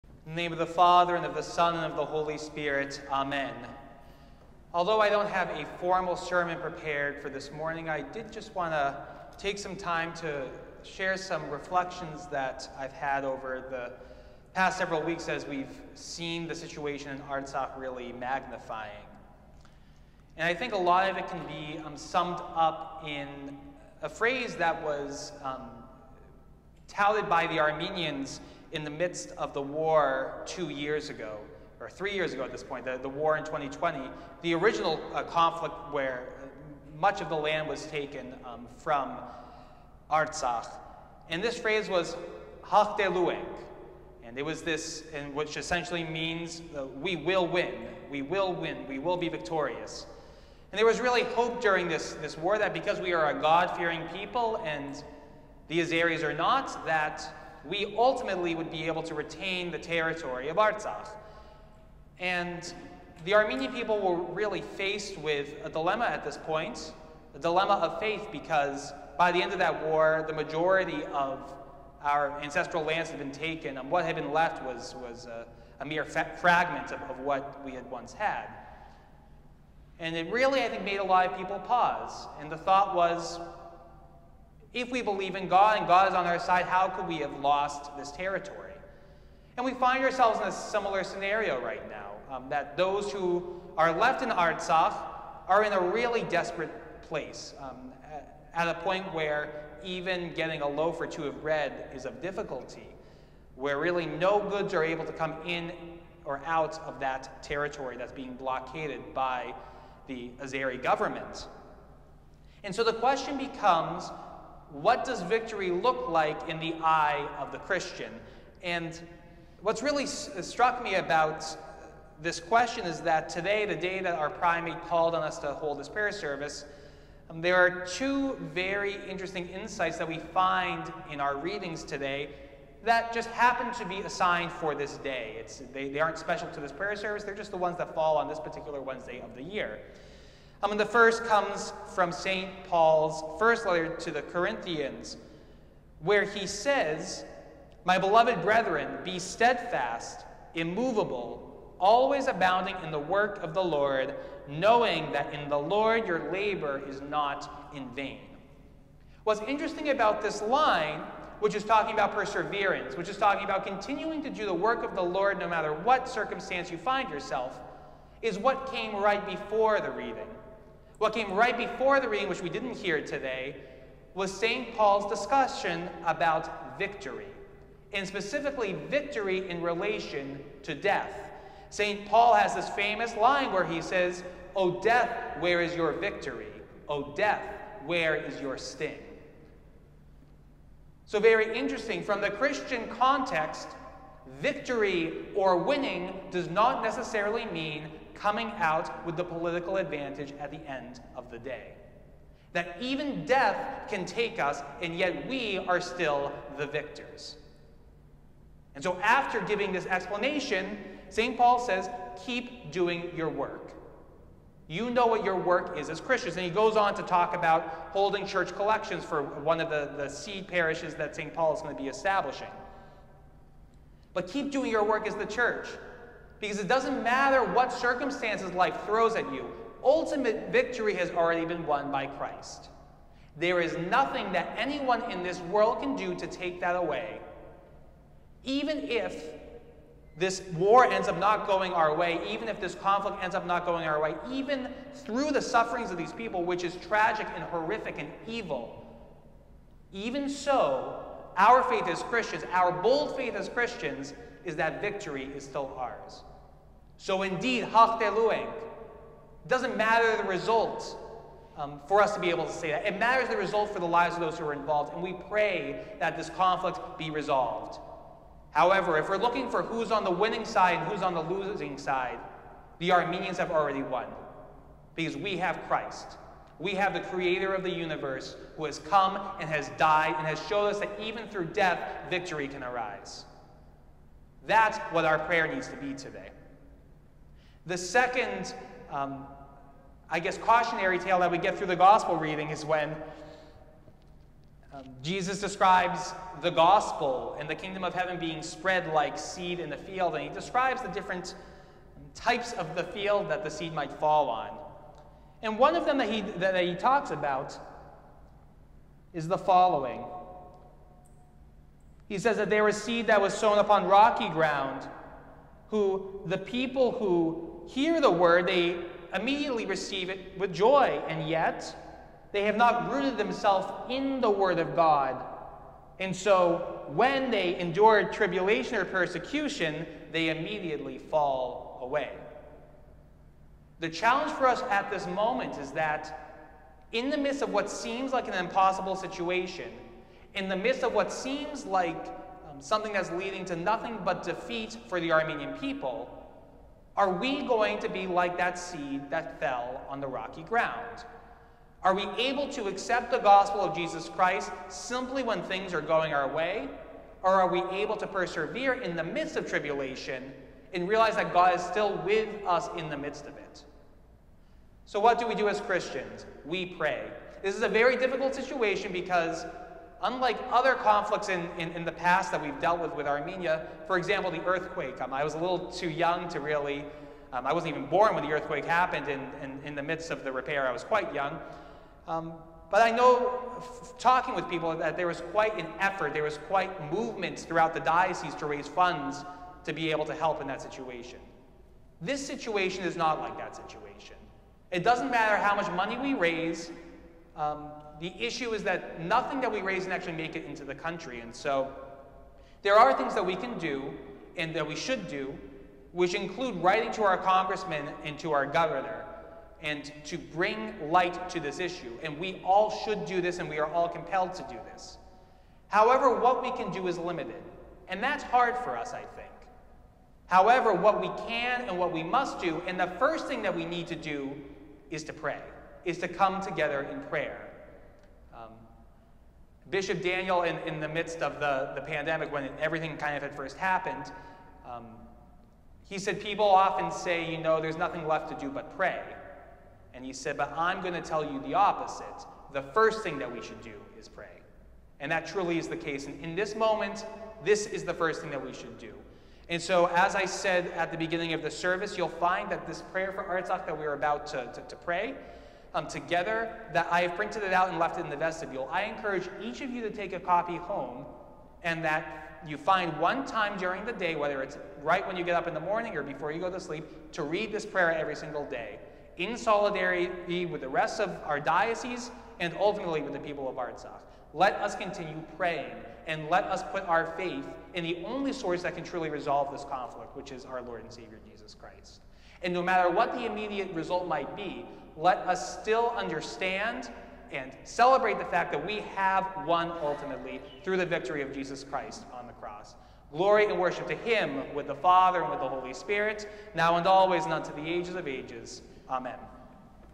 ACHR Sermon 08-23-23 - Special Prayer Service for Artsakh
For those who missed it, you can watch Fr. Haroutiun’s reflection during the special prayer service for Artsakh, during which he explored the true meaning of victory in Christ.